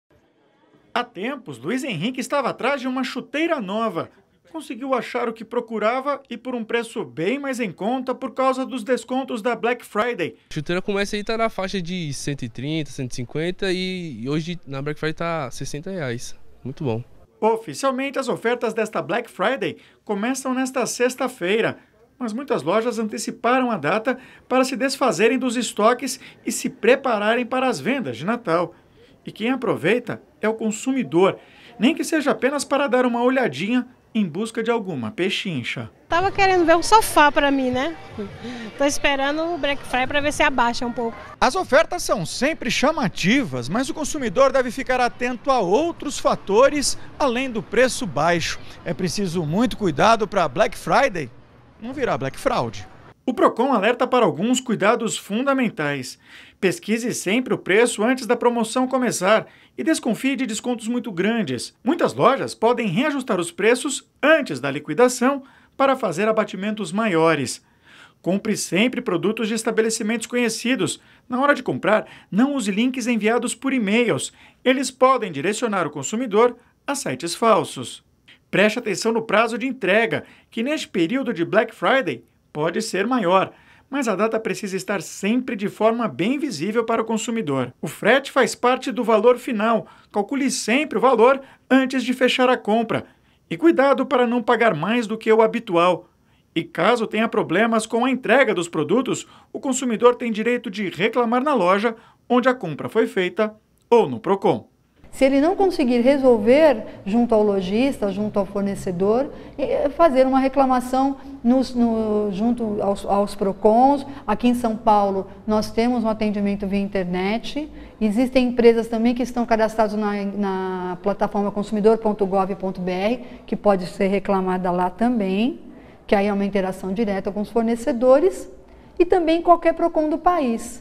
Semana Black Friday
REPÓRTER NBR - 22.11.18: Muita gente quer aproveitar as promoções para as compras de fim de ano! Mas a dica é ficar de olho na qualidade do produto, no preço e também na credibilidade da loja! Vamos ver na reportagem de Ricardo Ferraz.